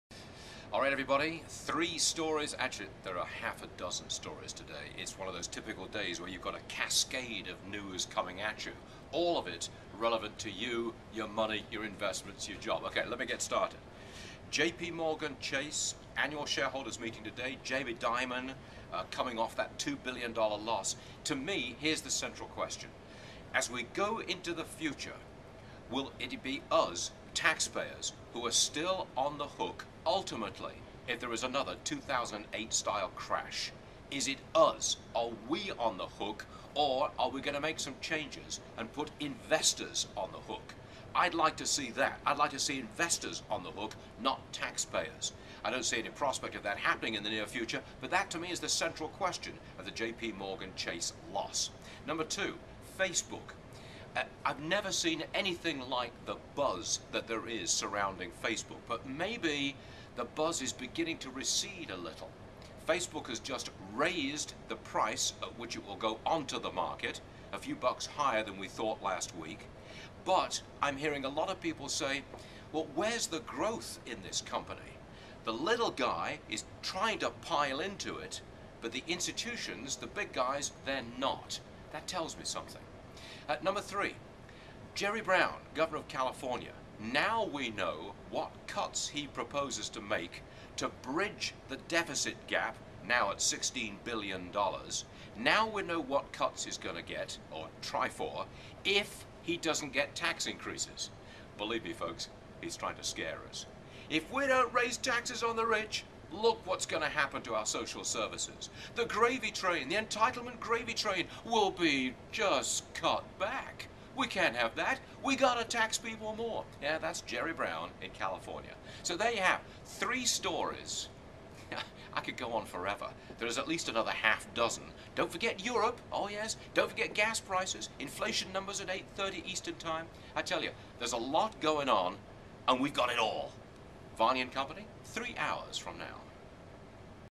JPMorgan, Facebook and Jerry Brown, Oh My!
There is too much news on this Tuesday morning to just talk about one story so Stuart covered three of the biggest.  We'll get to all of it this morning, starting at 9:20 ET.